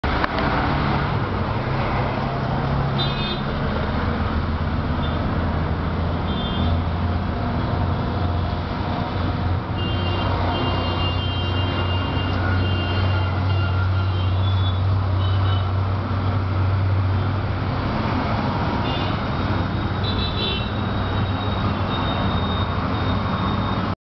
aedes mosquito knowledge
aedes/dengue/chicken gunya mosquitoes coming out of a drain in east of kailash.these tiny insects are seen as rapidly moving.this is the primary site of breeding of aedes mosquitoes.DRAINS ARE A MAJOR SOURCE.Eggs & larvae flow down to other parts of drain during rains thus spreading the breeding to far off areas.from drains ,the mosquito reach the desert coolers & not vice versa.